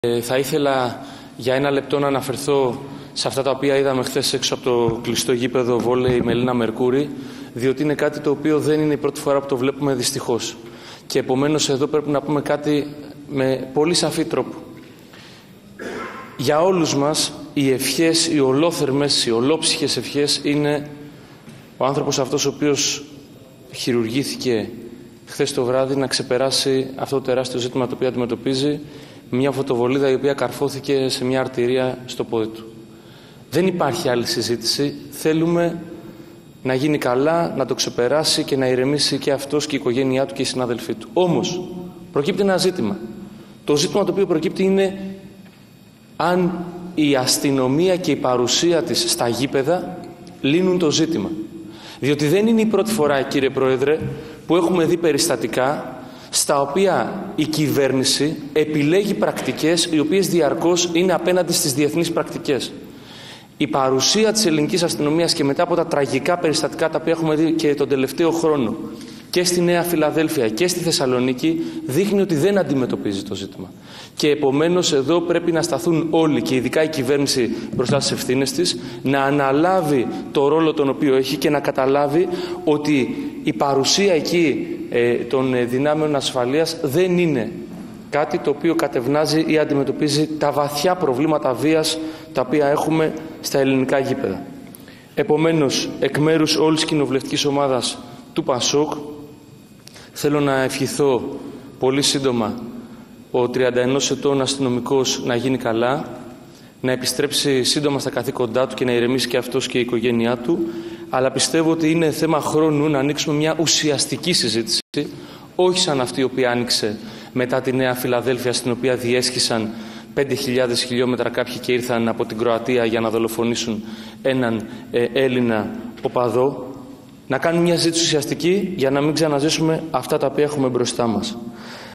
Η παρουσία της αστυνομίας δεν αντιμετωπίζει την οπαδική βία - Ολομέλεια Βουλής (8/12/2023)
«Η παρουσία των δυνάμεων ασφαλείας στα γήπεδα δεν κατευνάζει, ούτε αντιμετωπίζει τη βία στα ελληνικά γήπεδα».
Ολομέλεια Βουλής, Παρασκευή 8 Δεκεμβρίου 2023.